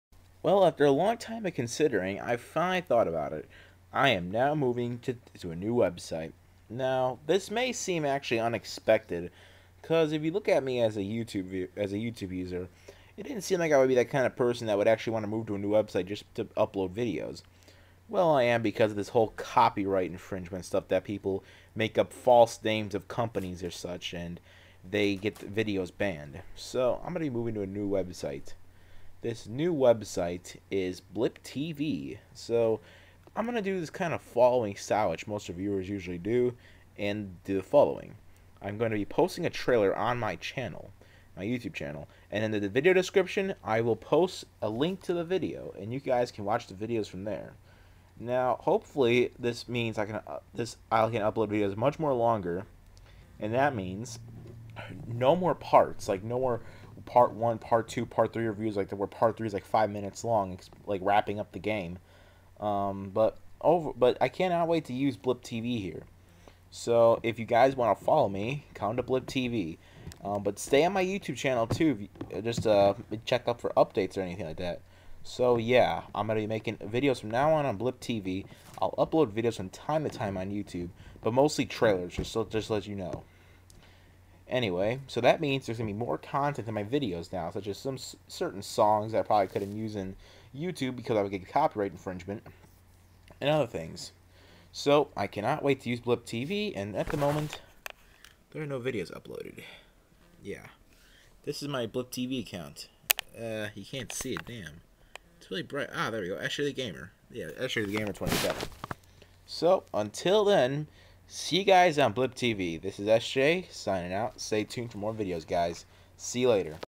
Moving to Blip TV for uploading videos. READ DESCRIPTION FOR LINK.
Thats right, not leaving permenately, just for uploading videos to Blip, Hope most of you will come and watch! Cant wait to see you guys there! Here is the link.

See ya guys! New reviews coming soon!